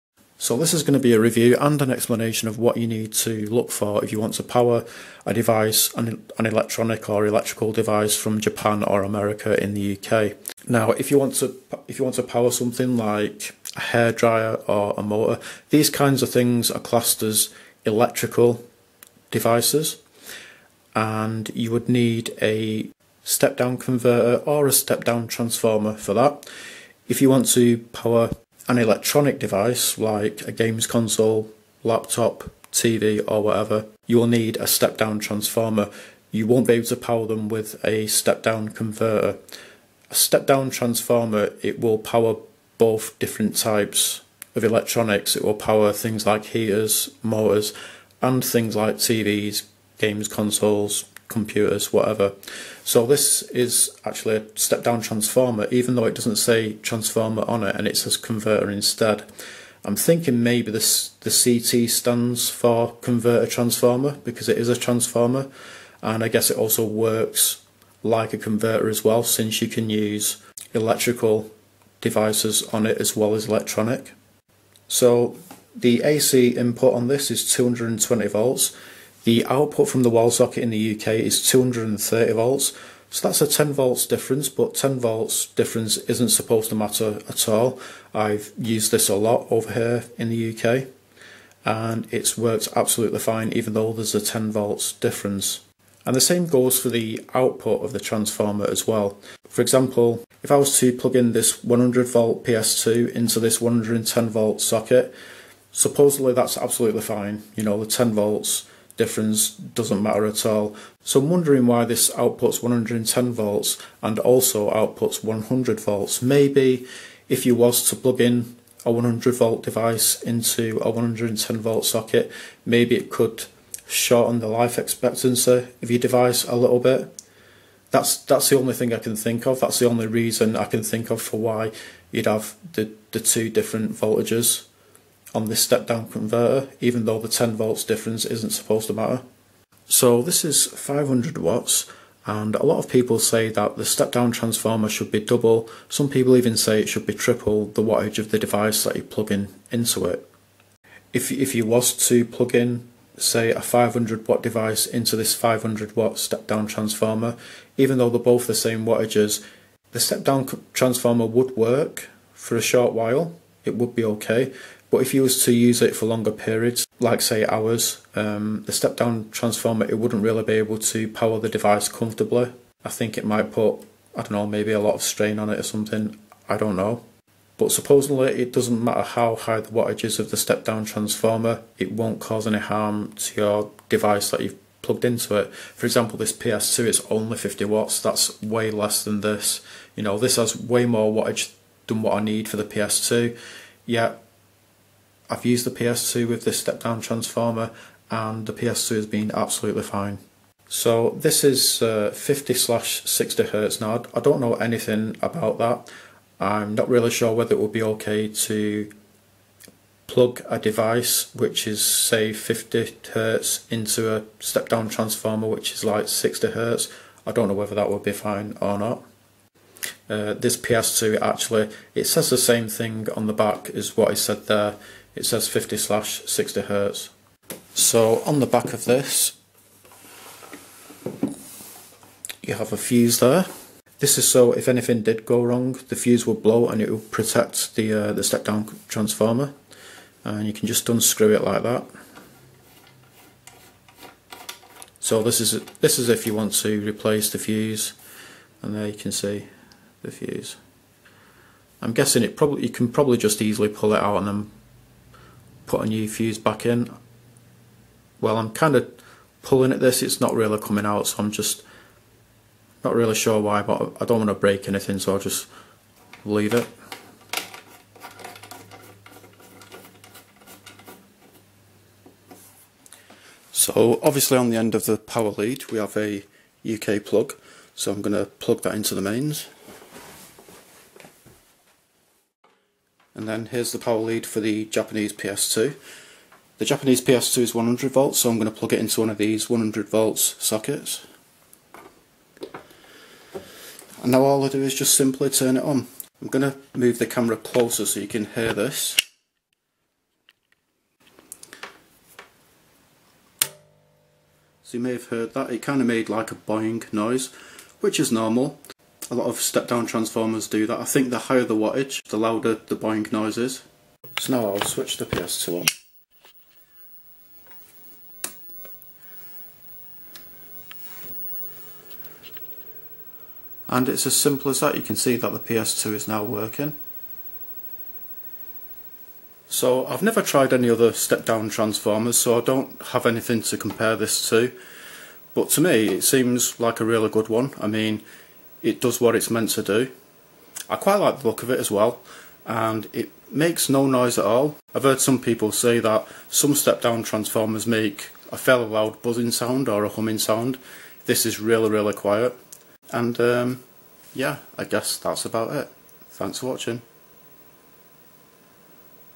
Super Star / Superstar CT-500 Review & What To Look For In A Step Down Transformer
In this video I review the Super Star / Superstar CT-500, and explain what you need to look for in a step down transformer.